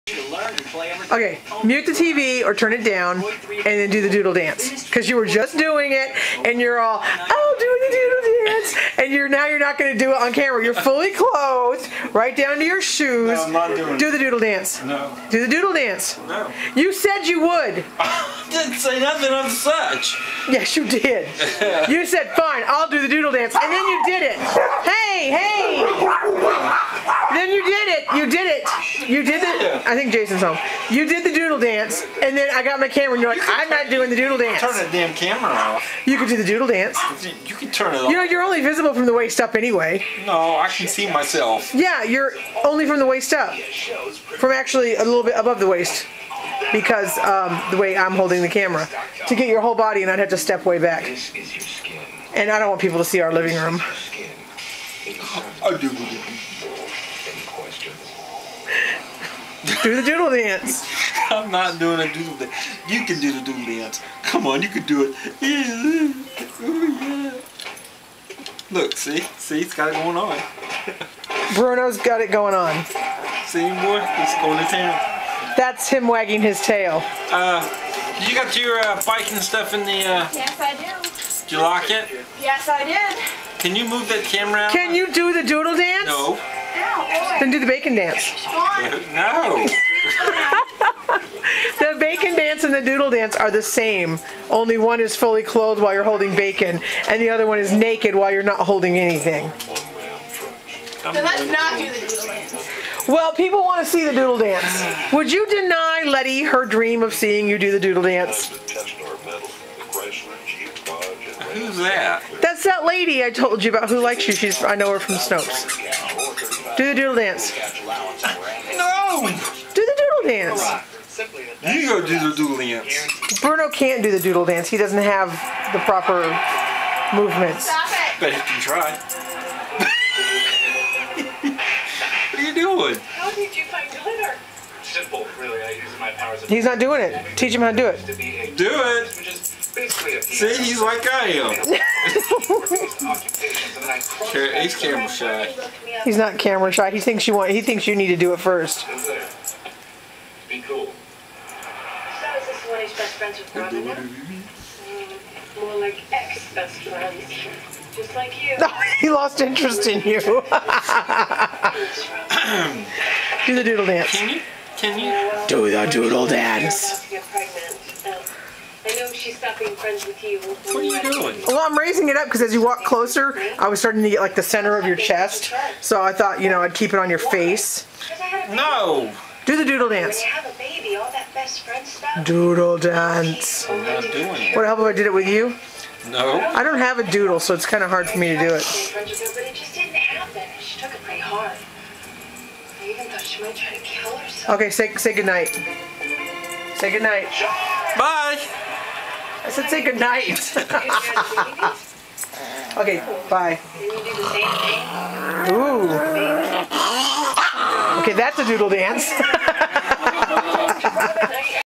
The Not-the-Doodle-Dance Video
There was discussion on the Facebook Group for Conversations with the Disabled Guy about the "doodle dance", related to the most recent blog post.

So, he stood up and was doing it, but stopped when I picked up my camera (he was fully clothed, so I have no idea why he stopped). This is the discussion that followed. What I learned from this video is that our stairway wall needs repainted. After me using it for support without a railing on that side, I've nearly rubbed the paint off and it looks dirty. (it isn't, it just isn't white paint like the rest of it).

So, we learned he won't do the Doodle Dance on camera, my stairway wall needs repainted, and Jasper is jealous of Bruno when the Disabled Guy is petting him.

Oh, and as soon as I turned the camera off, the Disabled Guy did a sped-up, screeching version of the Doodle Dance where he squealed: "Doodle-doodle-doodle-doodle!"